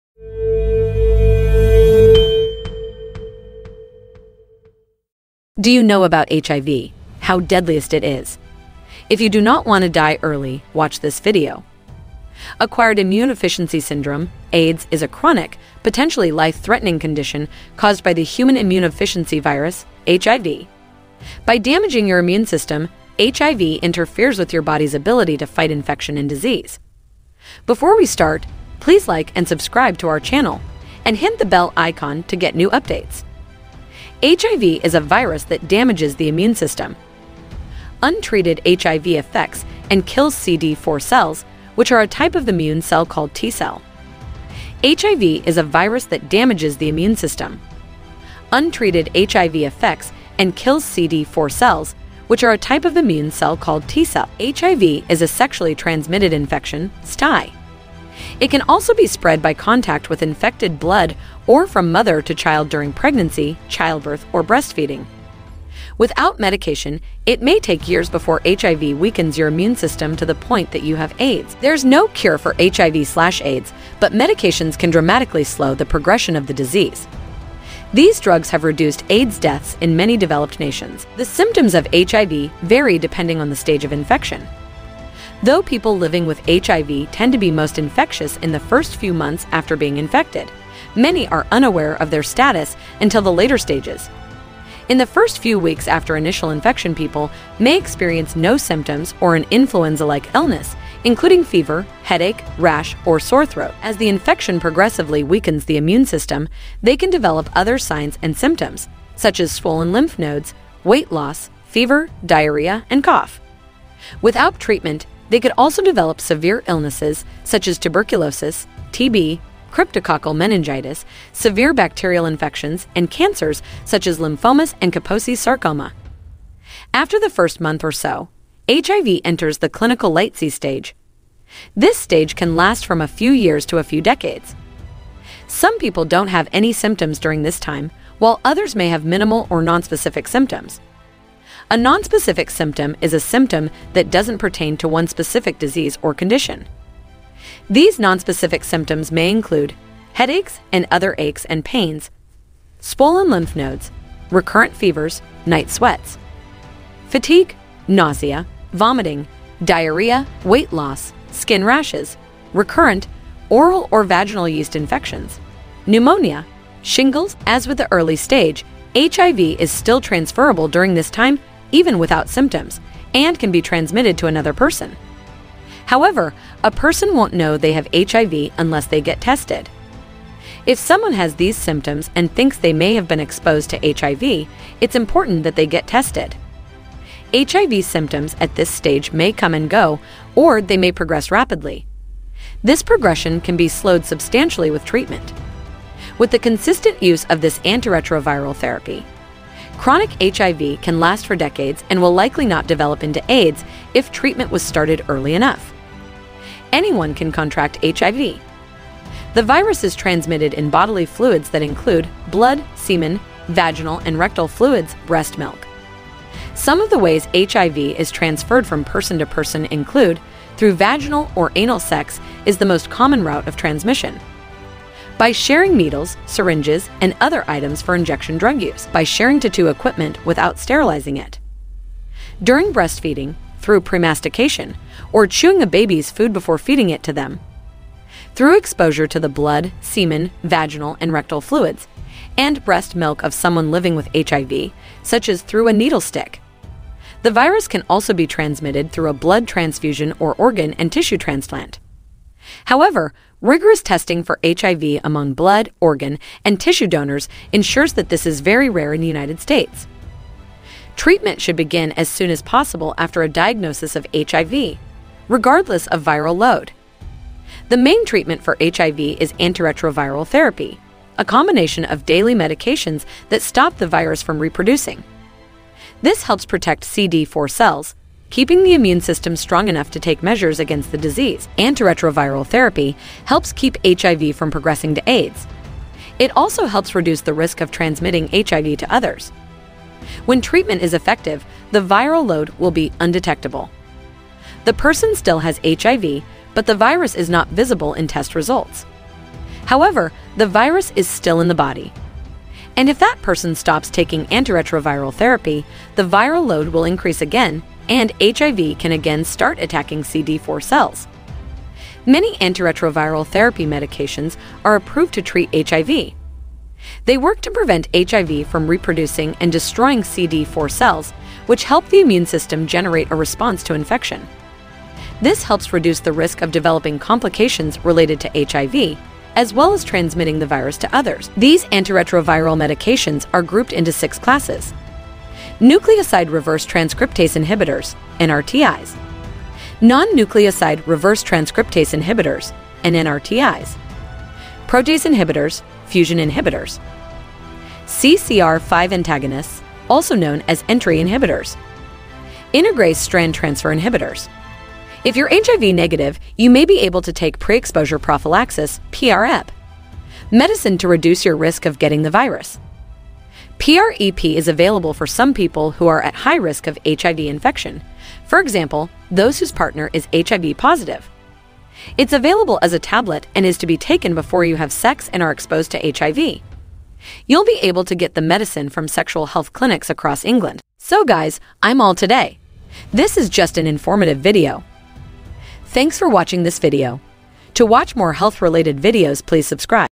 Do you know about HIV! How deadliest it is? | What It Means To Have Hiv
Do you know about HIV! How deadliest it is? | What It Means To Have Hiv | Why It’s So Hard To Cure HIV

Hey guys! Today I’m going to show you how to cure HIV. HIV is a virus that attacks cells in the immune system (the body’s natural defense against illness). The virus destroys a type of white blood cell in the immune system called a T-helper cell – also referred to as a CD4 cell – and uses these cells to make copies of itself. You can learn also about HIV, aids, HIV nursing, aids nursing, Health Tips, Health Coach

                       ★★ SOCIAL LINKS ★★

Hiv, Aids, HivNursing, AidsNursing